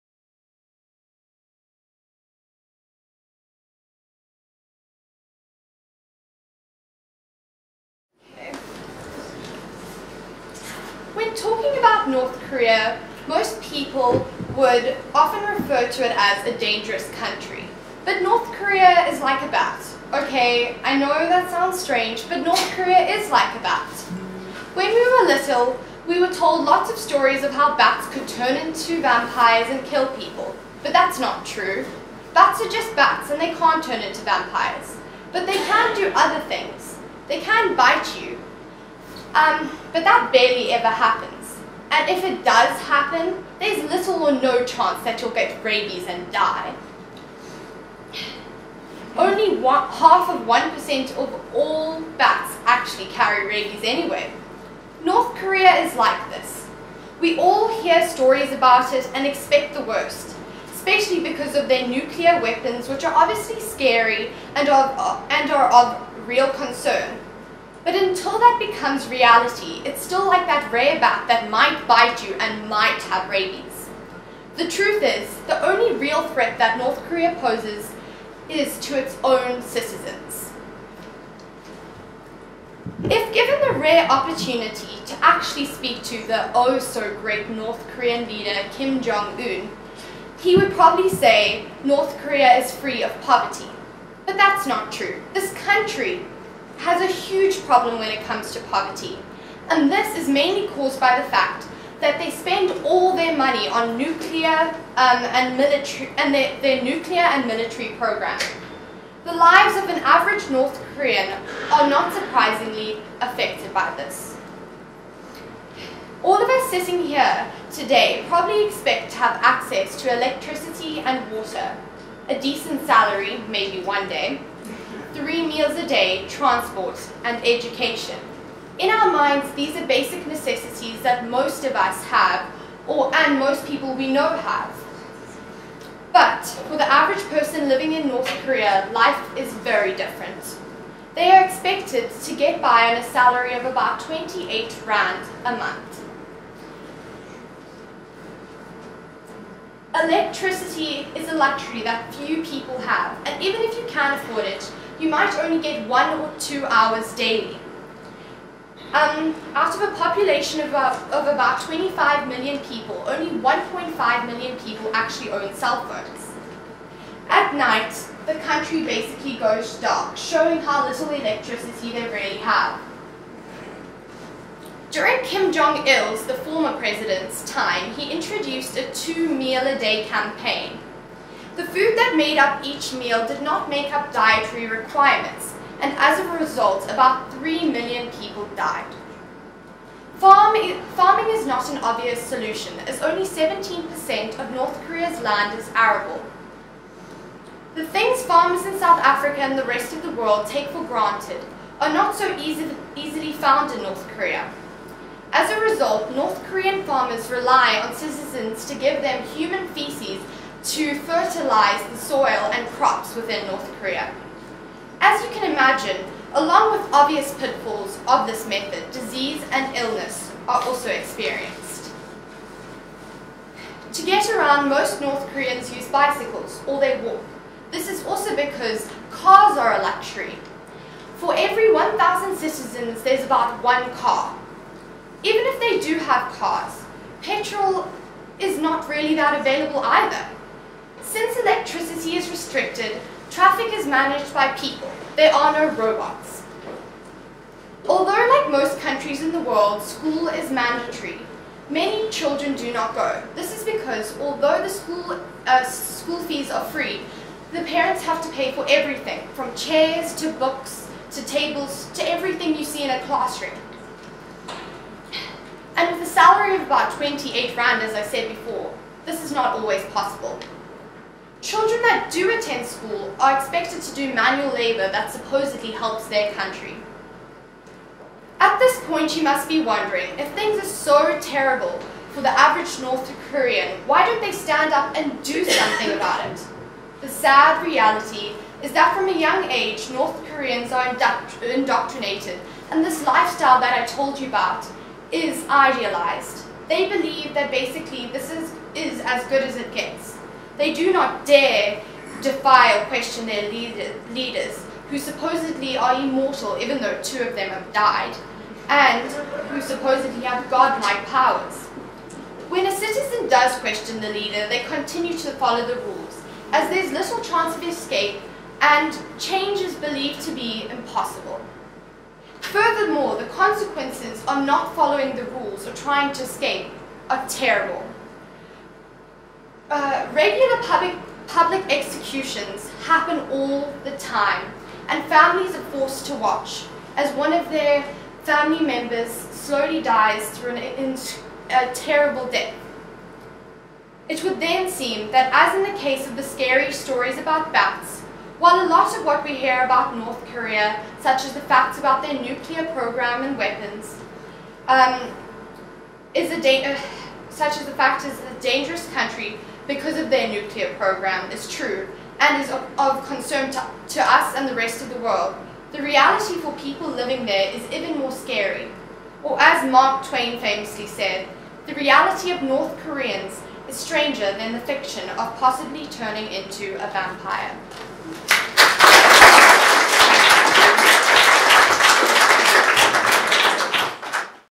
TED Ed Club - Caitlin Murphy: North Korea: The people's story
Caitlin was part of the first TED-Ed Club in South Africa - presented by Think Ahead - and this is her talk on North Korea.
The TED-Ed Club program supports students in discovering, exploring and presenting their big ideas in the form of short, TED-style talks.
Caitlin is a student at Roedean Girls' School in Johannesburg.